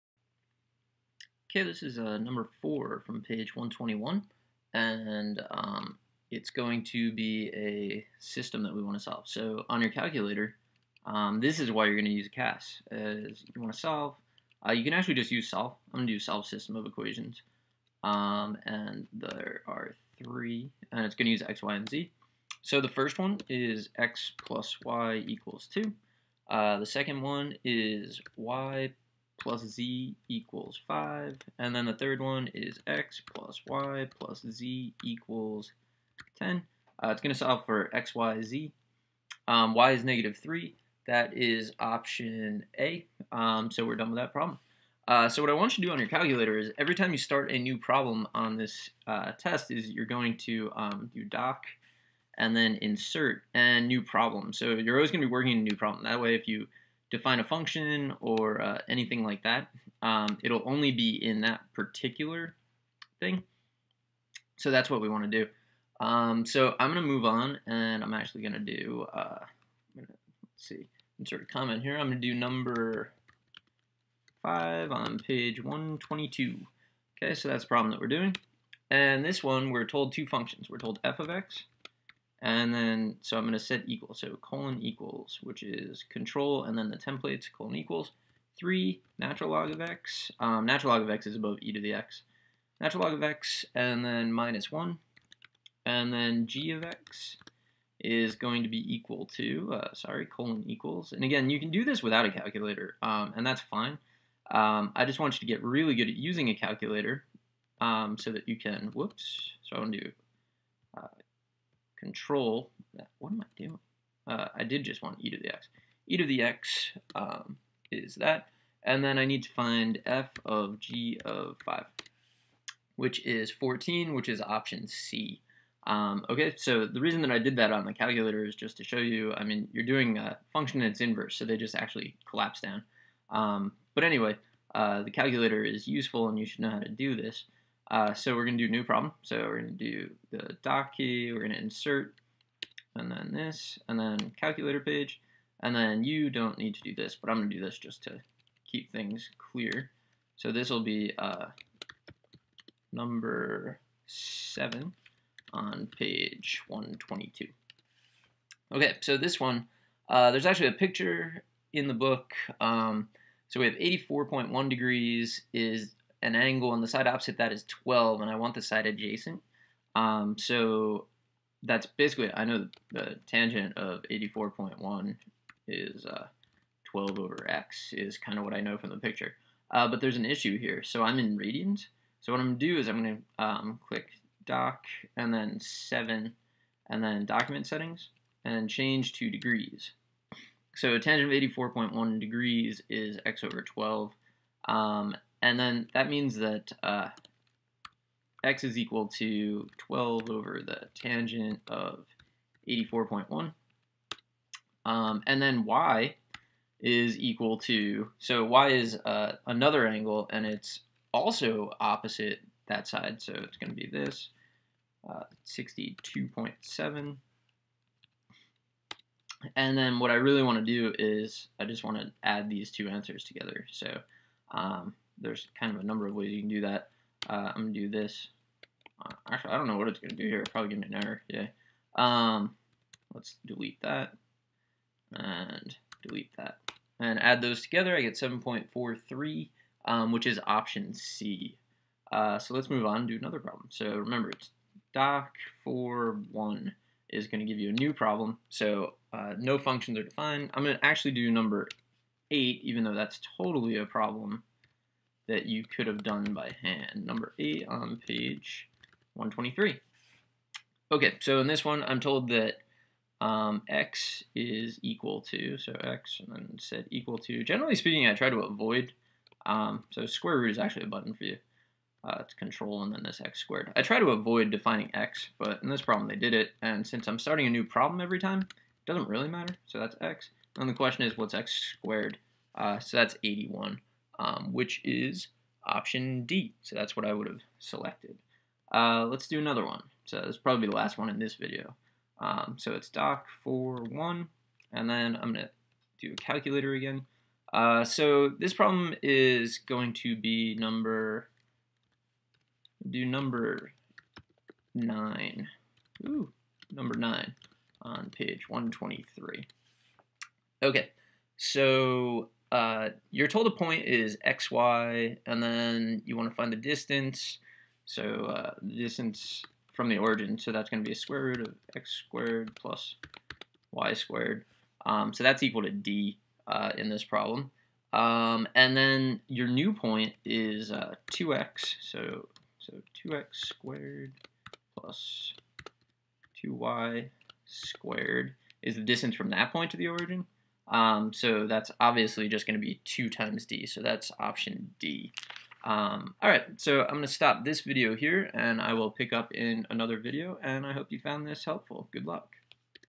SAT II Math 2 Blue Book Practice Test 1 By Calculator (Part 1)
Using a TI-Nspire CAS to do as many problems on the SAT II Math II as possible.
Numbers 4, 5, 7, 8, 9

0:00 Problem #4
1:12 Problem #5
2:30 Problem #7
4:12 Problem #8
4:59 Problem #9